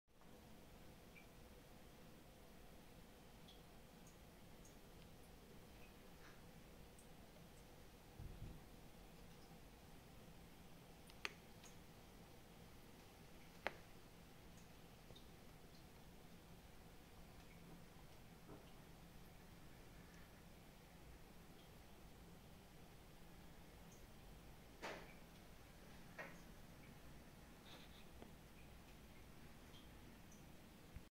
Charle Shallowreef SPS aquarium based on Aquaforest products
This very interesting scape and beautiful corals always has been a big pride among the wonderful tanks based on Aquaforest products. Let's take a closer look at this.
Join the group and become a part of growing Aquaforest community